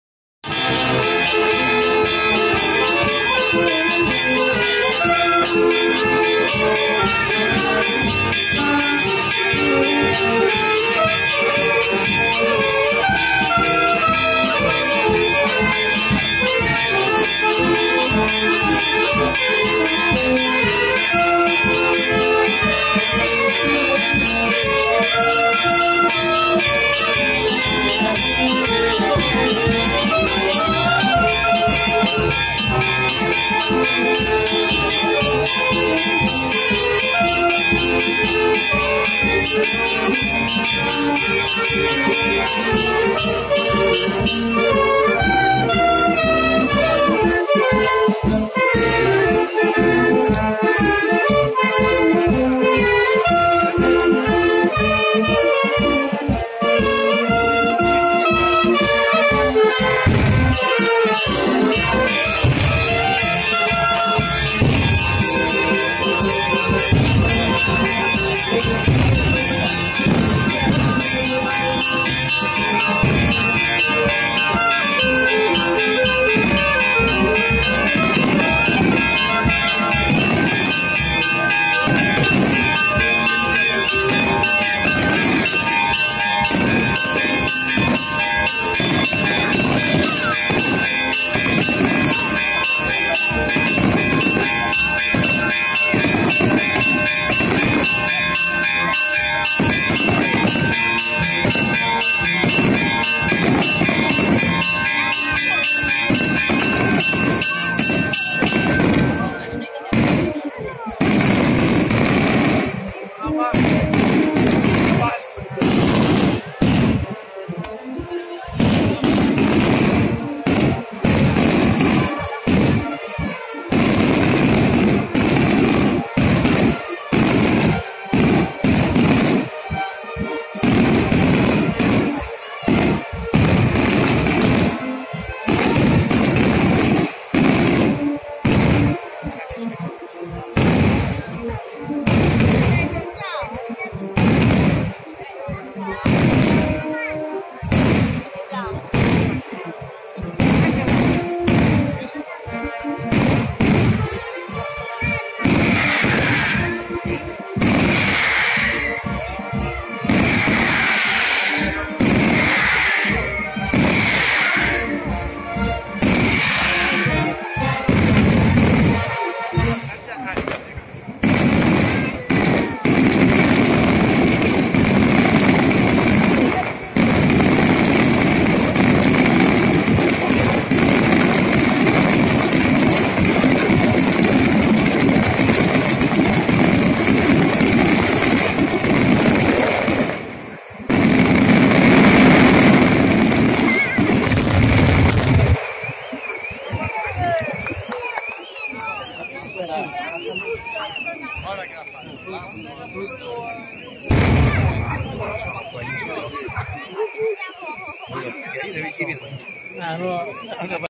ACI BONACCORSI,26 DICEMBRE 2019 - SANTO STEFANO PROTOMARTIRE
Uscita del Simulacro di Santo Stefano